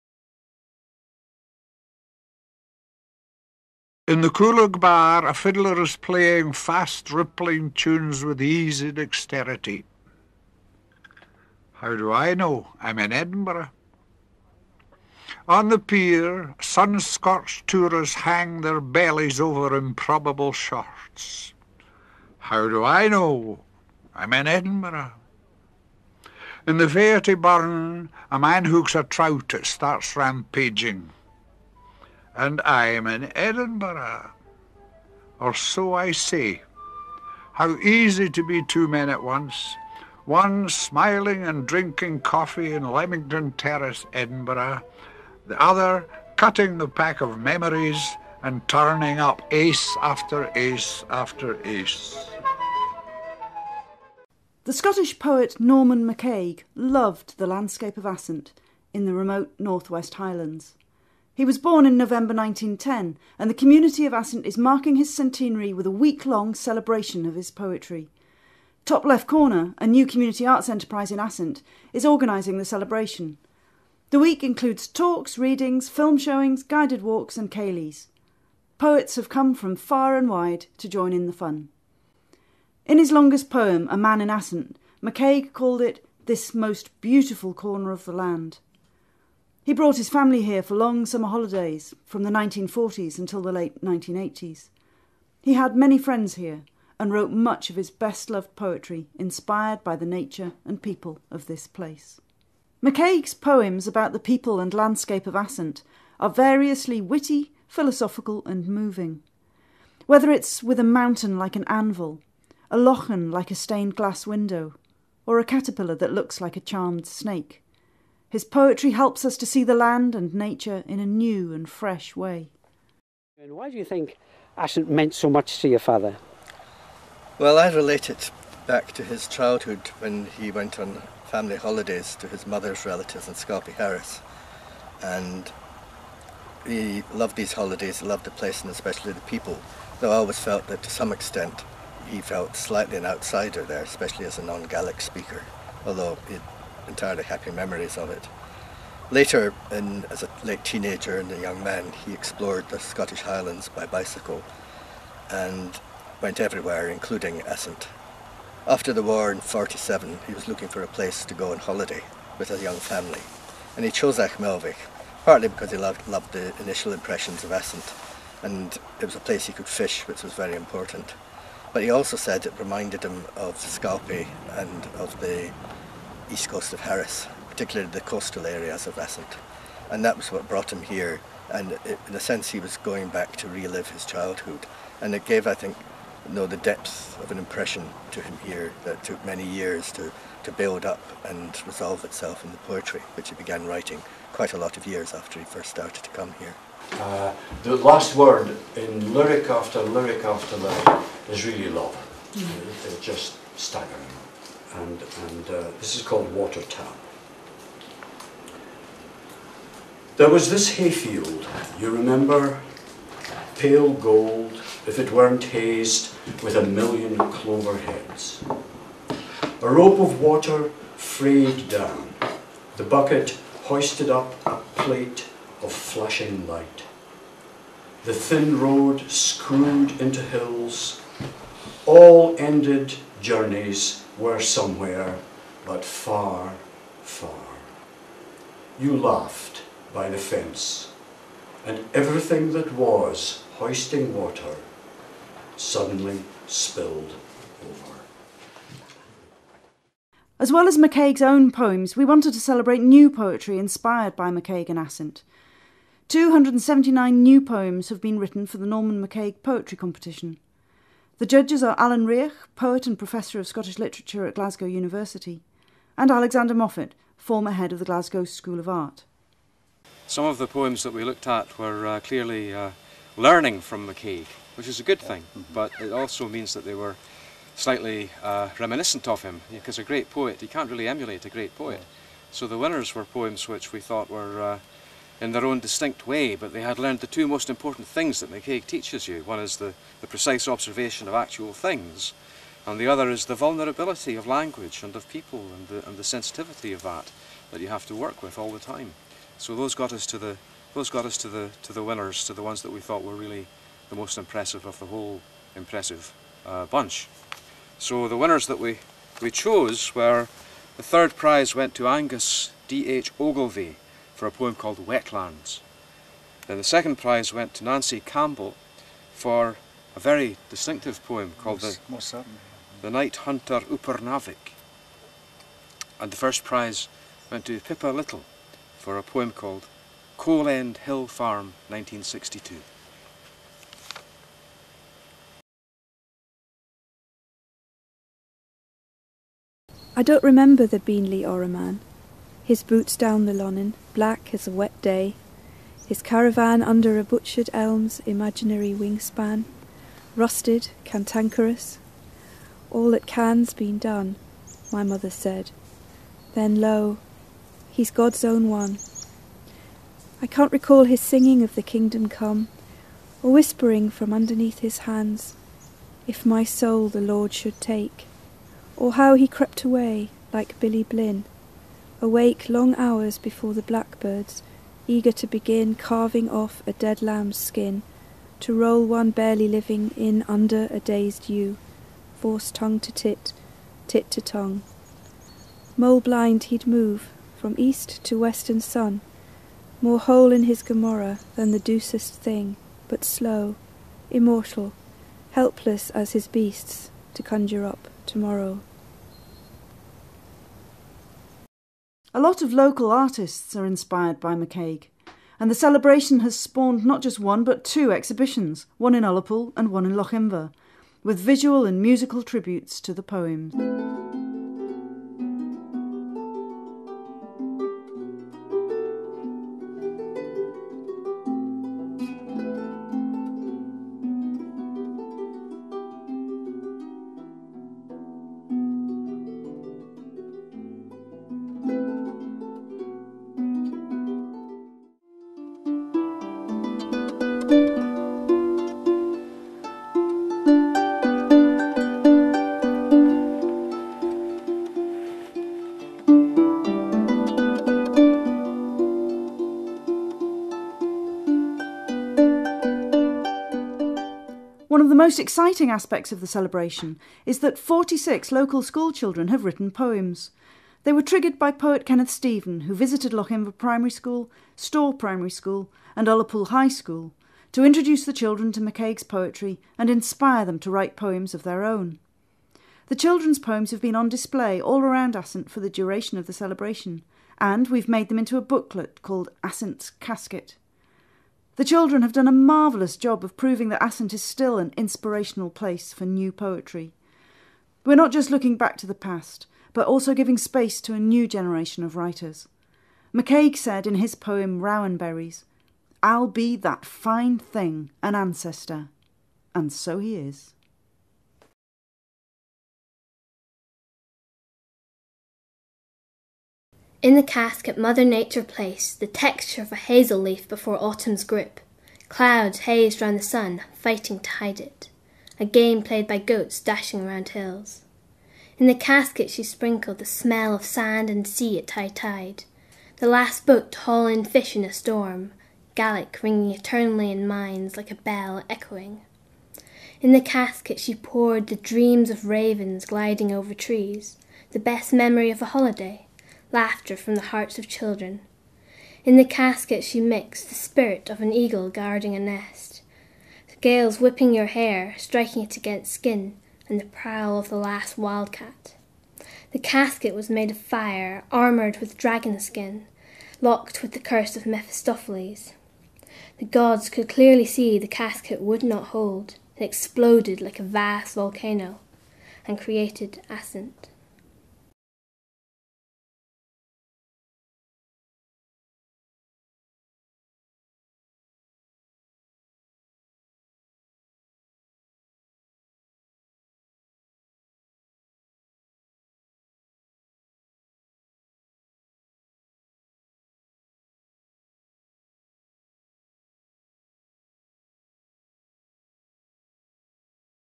Norman MacCaig: A Man in Assynt
Highlights from a week long celebration of the centenary of the Scottish poet Norman MacCaig by the people of Assynt and friends.